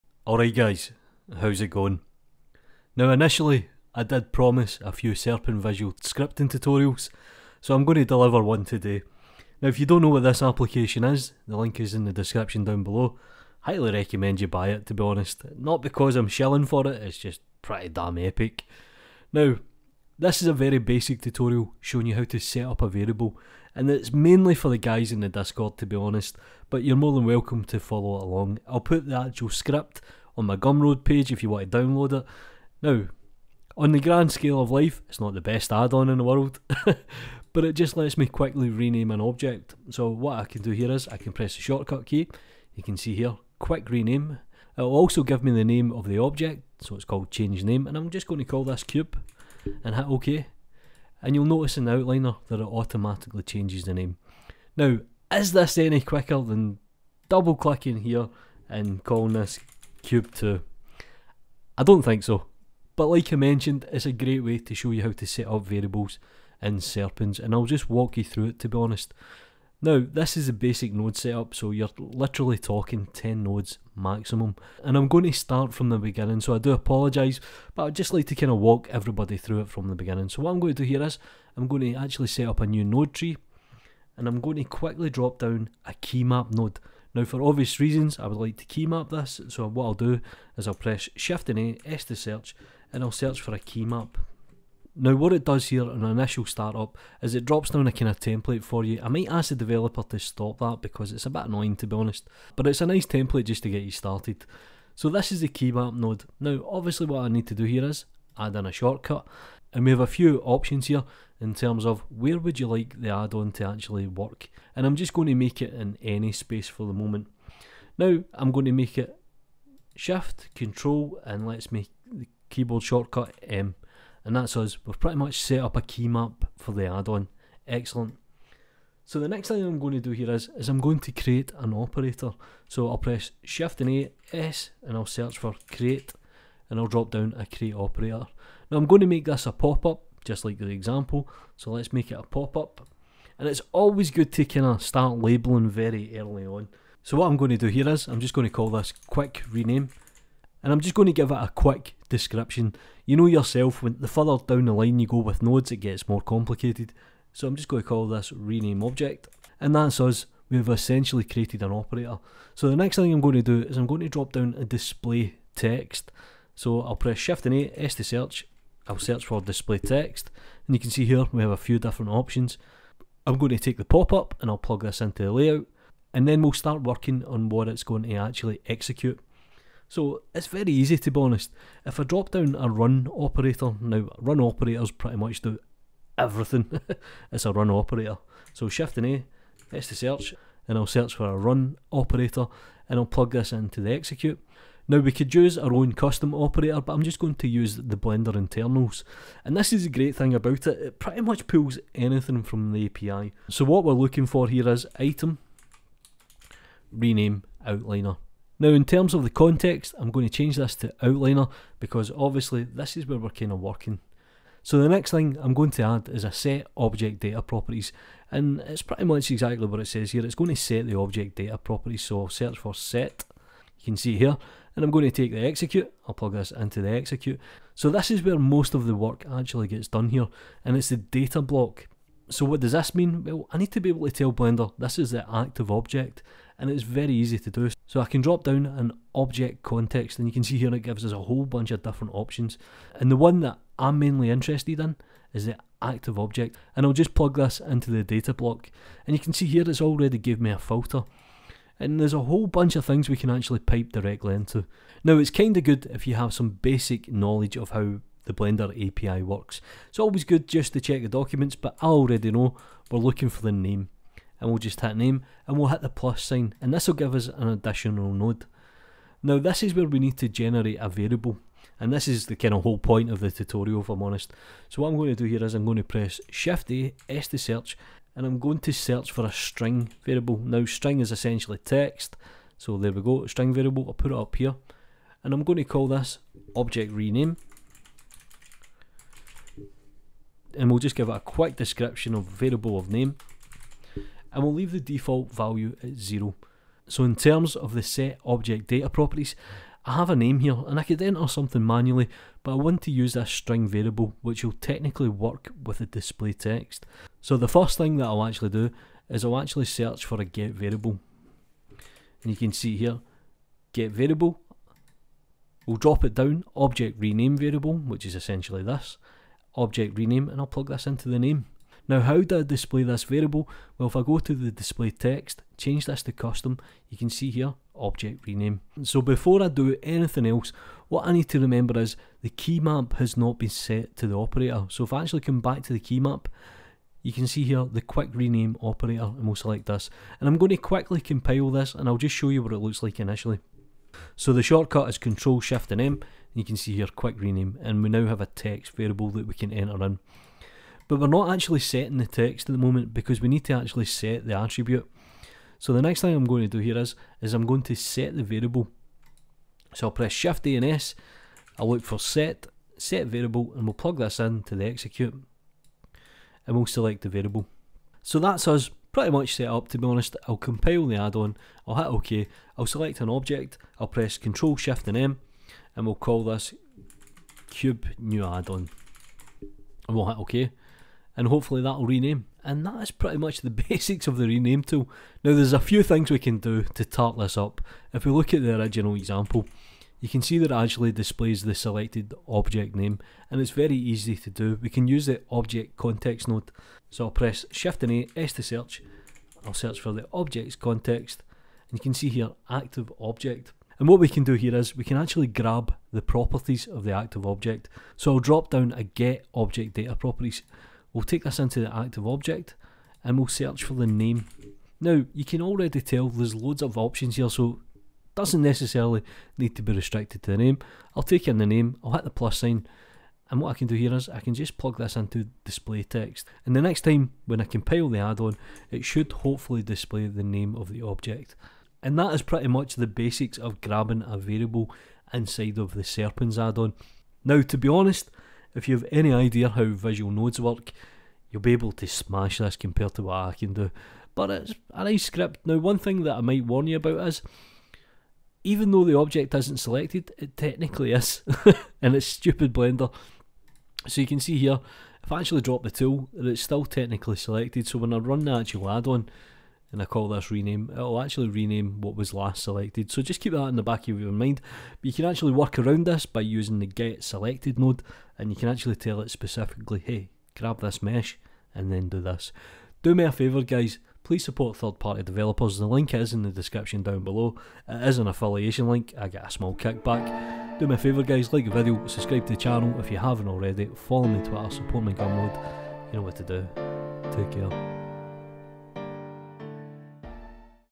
Renamer addon - Serpens Visual Scripting get string variable tutorial
Step by step how to create a rename addon using serpens Visual Scripting